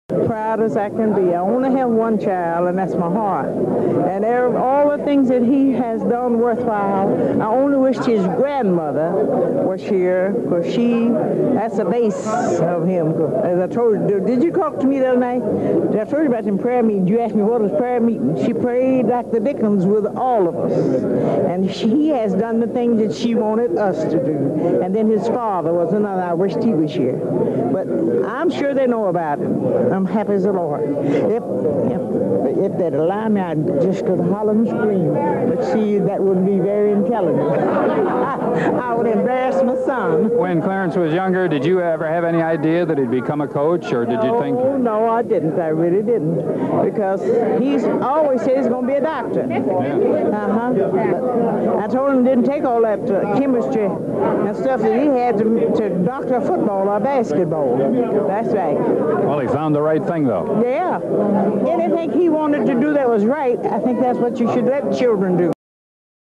Miss Ola Speaks
Olivia Bolen Gaines talks about her son, Clarence "Big House" Gaines, on the day (1982) he was inducted into the Naismith Memorial Hall of Fame.